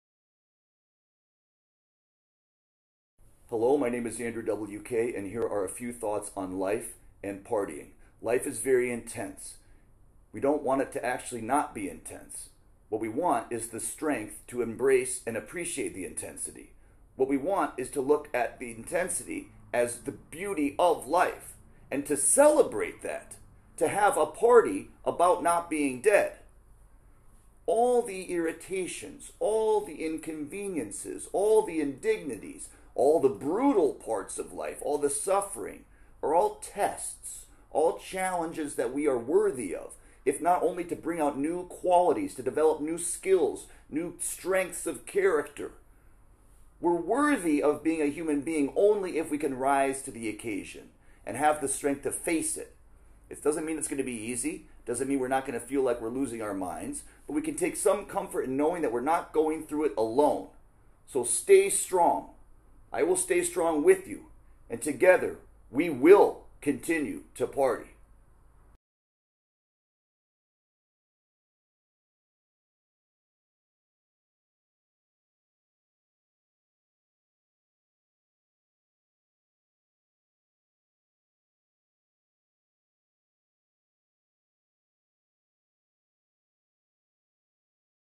Andrew W.K.'s Life Lessons: Life And Partying
Listen to Andrew W.K.'s epic advice on living your life.